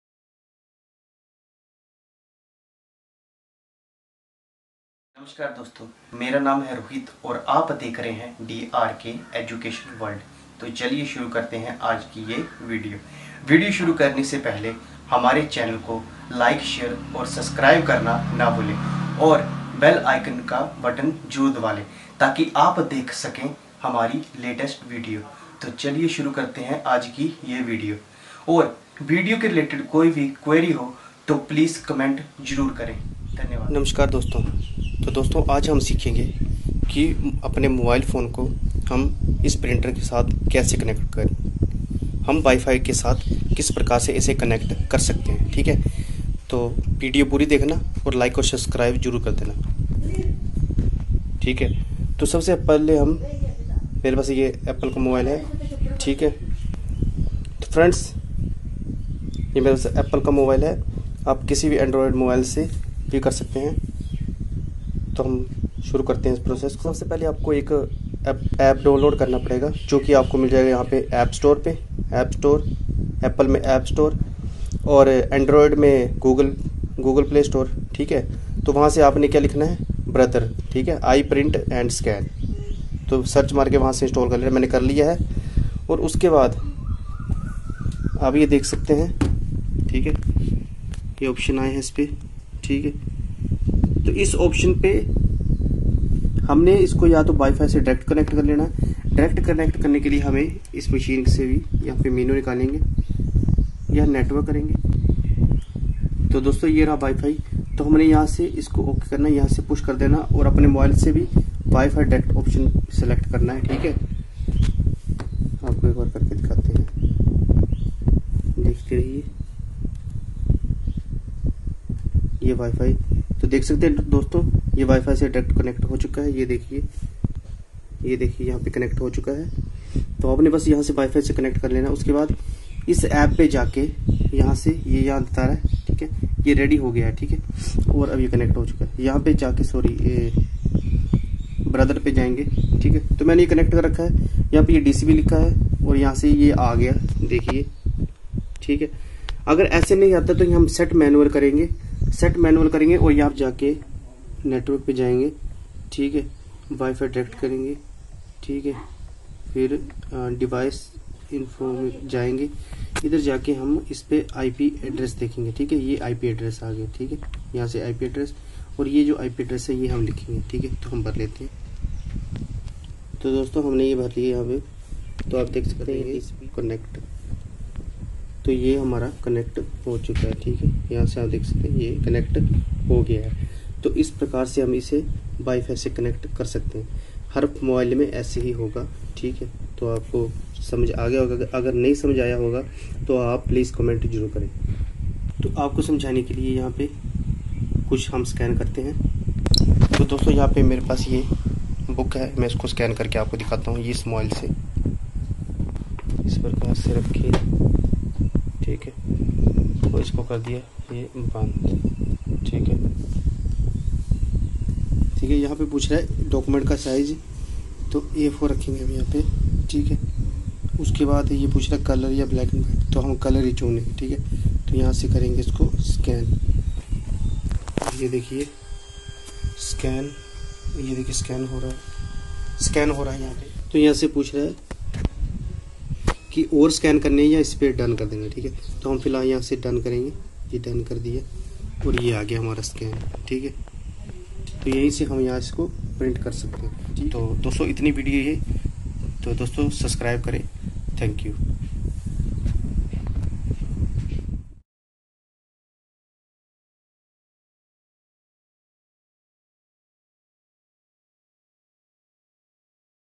Brother DCP-T510W USB-WI-FI Printer Quality Connect to WIFI Mobile
BROTHER DCP-T510W INK TANK PRINTER

How to connect your Mobile and Printer with wirelessly or without wire using Wi-Fi in Android Mobile and Apple Mobile
 Software Downloads :-

In Android

Open Play store and search

Brother iprint and scan

In Apple

Open App store and search

 ALSO COMMENT